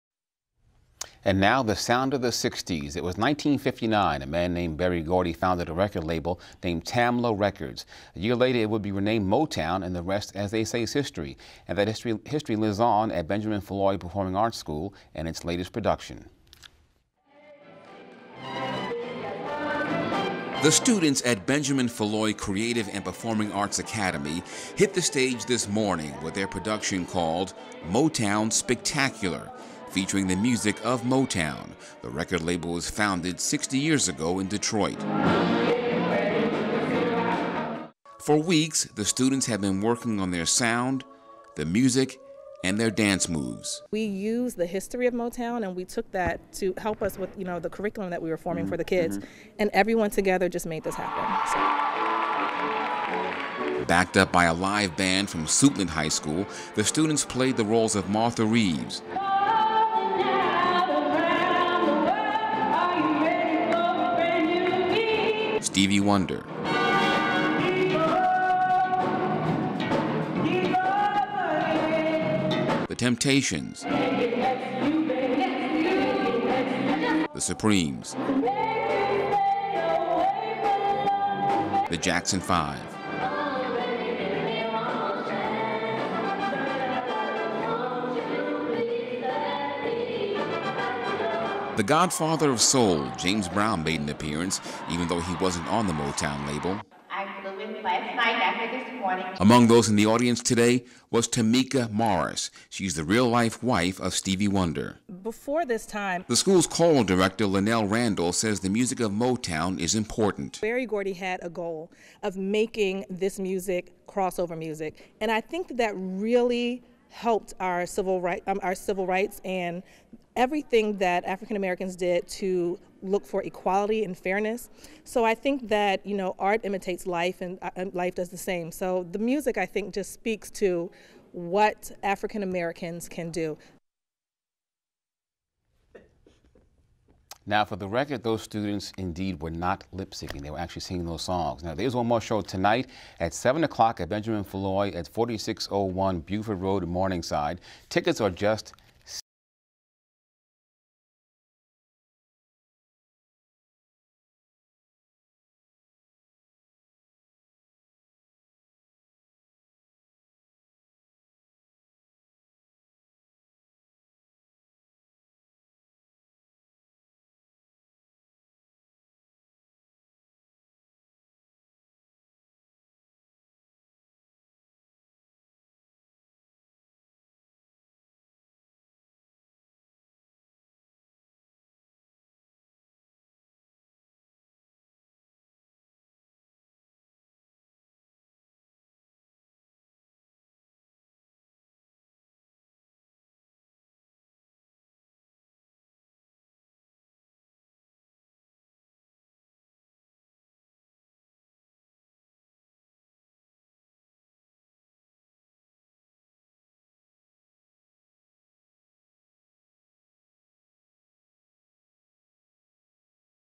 Motown Spectacular II
The Benjamin Foulois Creative and Performing Arts Academy performs Motown Spectacular before a packed house. The stage play honors the 60th anniversary of Motown music.